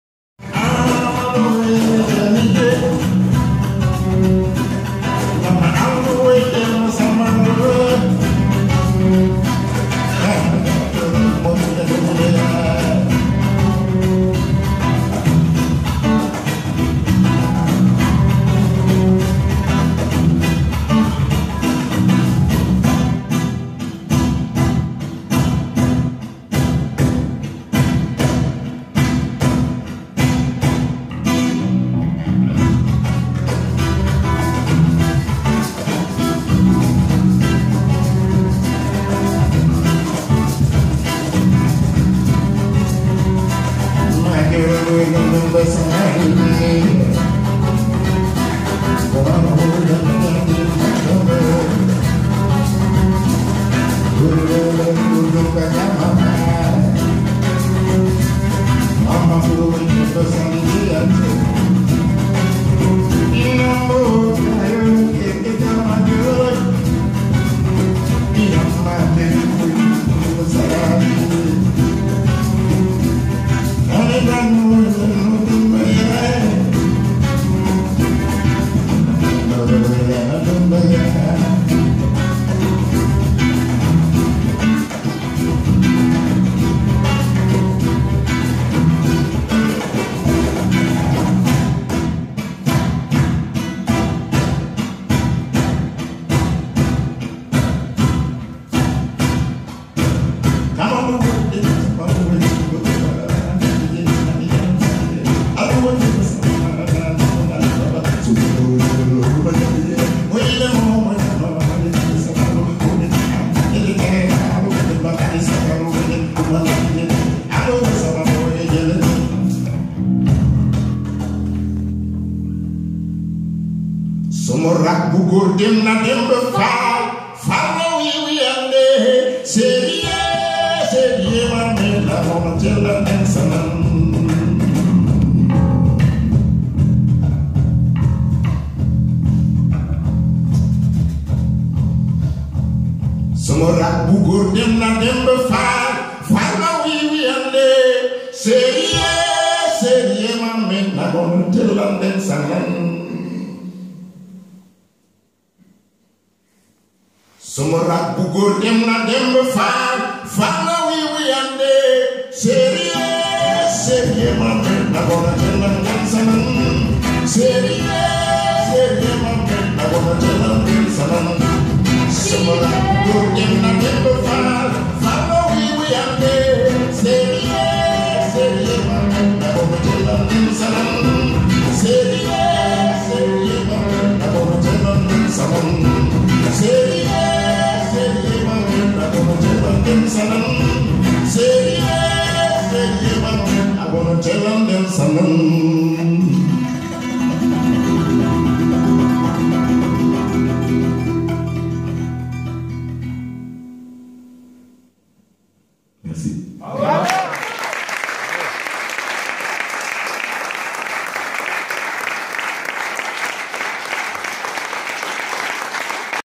(13) CONCERT DE SOUTIEN AUX ENFANTS DU NÉPAL
Concert de soutien aux enfants du Népal, ancien Carmel de Moissac le 15 octobre 2016. SYLVIE ENA ( VIOLONCELLE ) SOPHIELOU ( CHANT ) PAAMATH N'DIAYE ( CHANT, GUITARE ) RICHARD ARRILLAGA ( BASSE ) CHRISTOPHE SETIAN ( CAJON ). Concert organisé par ISABELLE HUC et GUY ENA pour LES ENFANTS DU TOIT DU MONDE.